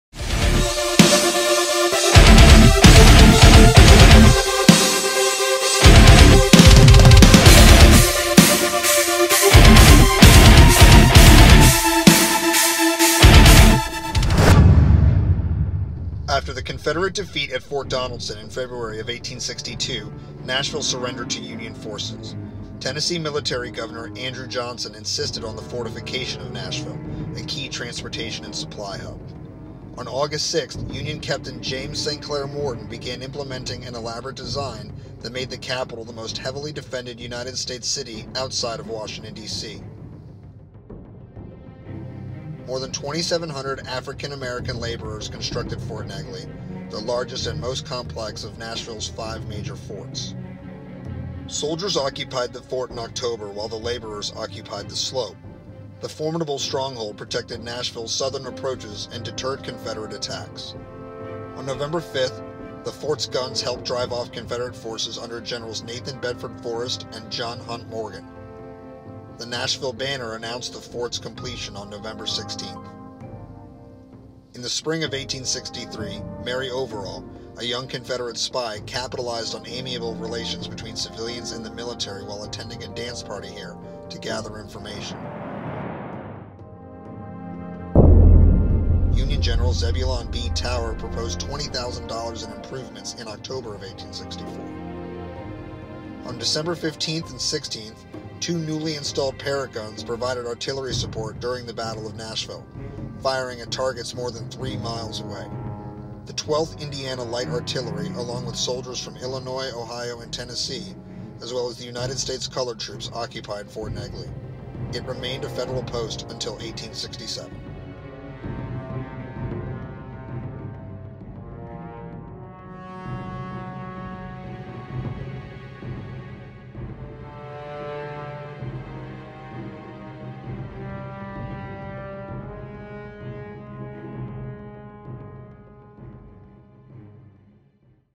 Civil War Trails: Fort Negley - Nashville, Tennessee
History Things with Pat narration of the Civil War Trails sign located at the entrance to Fort Negley in Nashville, Tennessee!